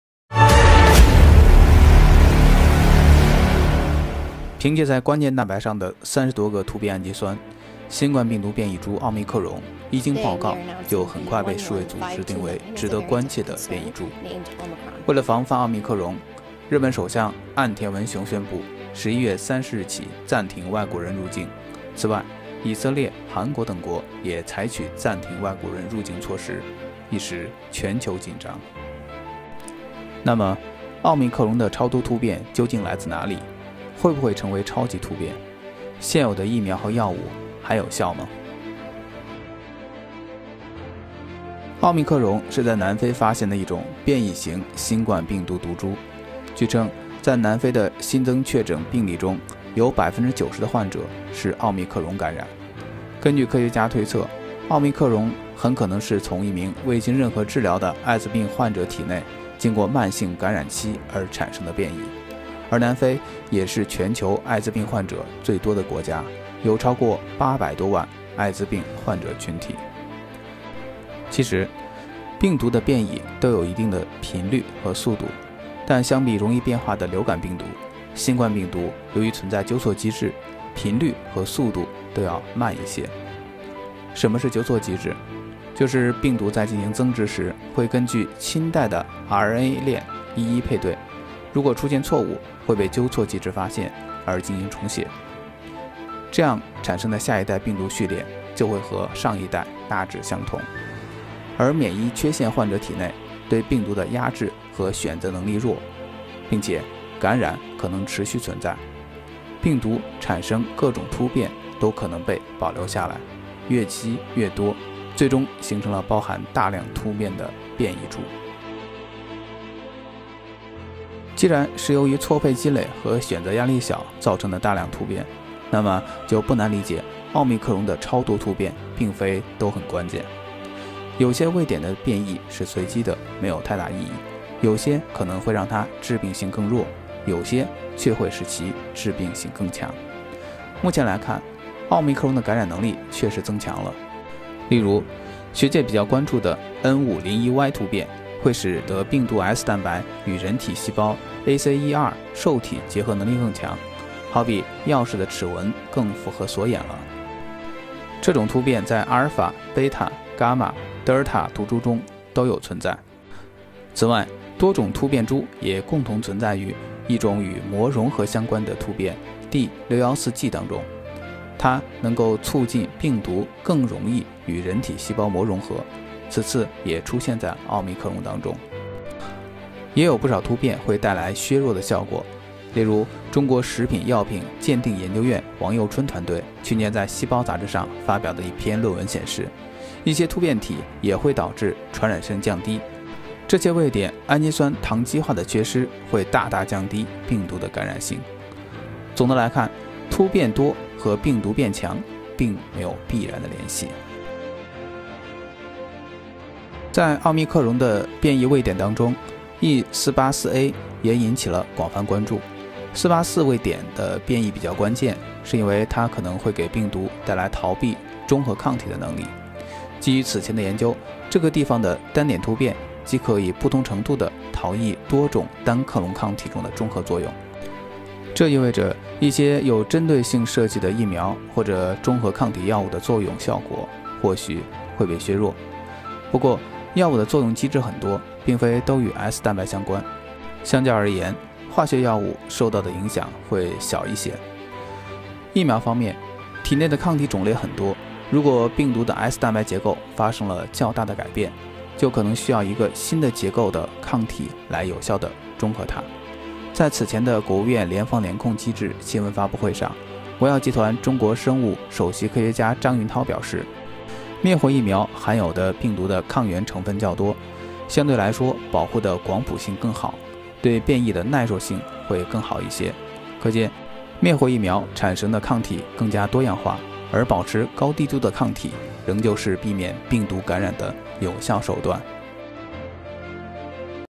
新冠病毒变异株“奥密克戎”为何让全球紧张？现有育苗对它还有效吗？
凭借在关键蛋白上的30多个突变氨基酸，新冠病毒变异株“奥密克戎”一经报告，很快被世卫组织定为“值得关切的变异株（VOC）”。一时间，全球紧张。那么，“奥密克戎”的超多突变究竟来自哪里？会不会成为“超级突变”？现有疫苗、药物还有效吗？这条视频来回答你疑问。